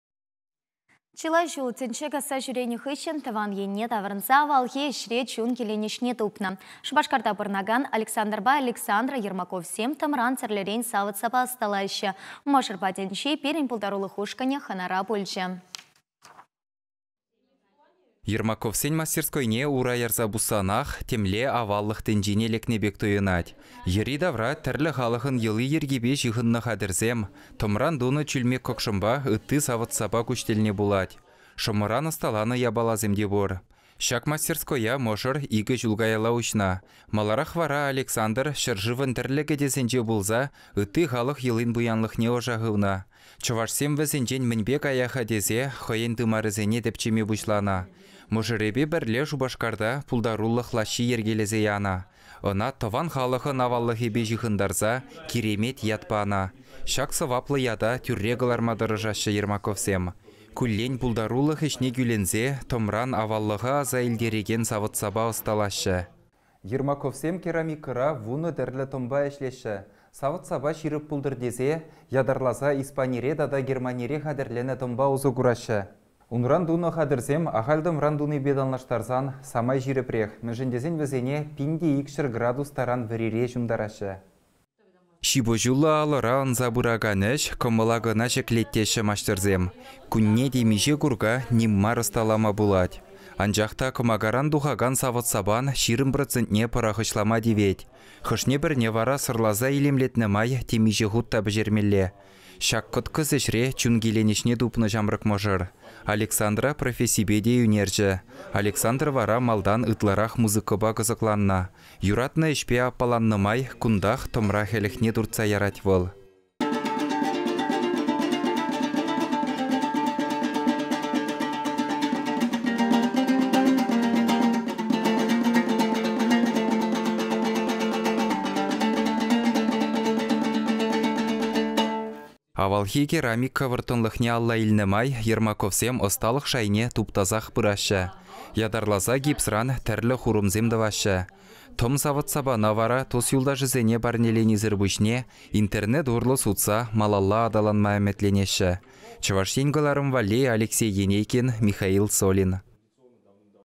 Шупашкарта пурӑнакан ҫамрӑк мӑшӑр тӑмран тӗрлӗ савӑт-сапа ӑсталать
Чылай ҫул тӗнче касса ҫӳренӗ хыҫҫӑн тӑван ене таврӑнса авалхи ӗҫре чун киленӗҫне тупнӑ. Шупашкарта пурӑнакан Александрпа Александра Ермаковсем тӑмран тӗрлӗрен савӑт-сапа ӑсталаҫҫӗ.